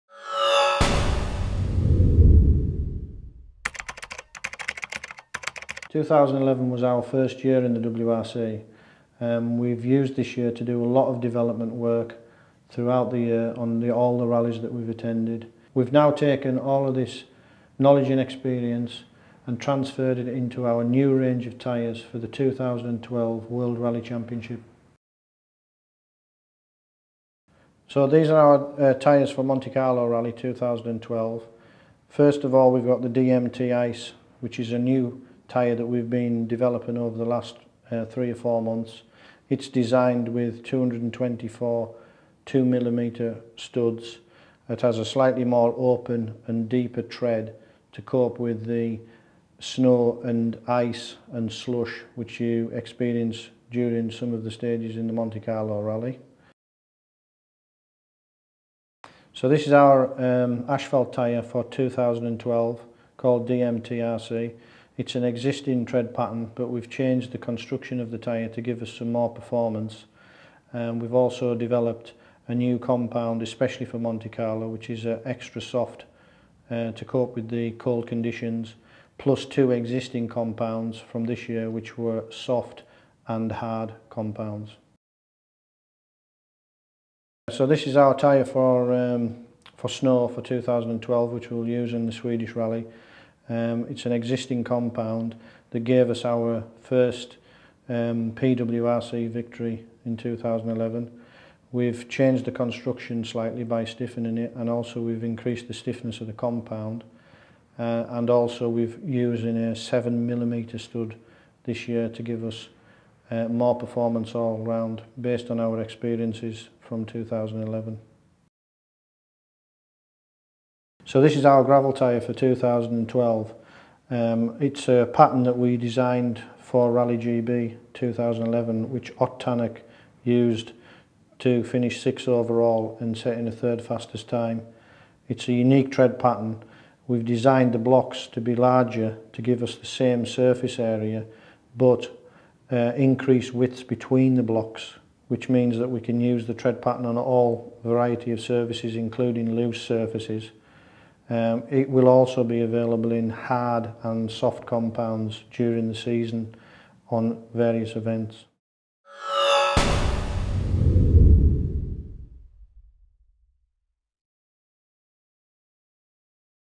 DMACK Tyres 2012 WRC Tyre Range
DMACK Tyres has launched is range of rally tyres for the 2012 FIA World Rally Championship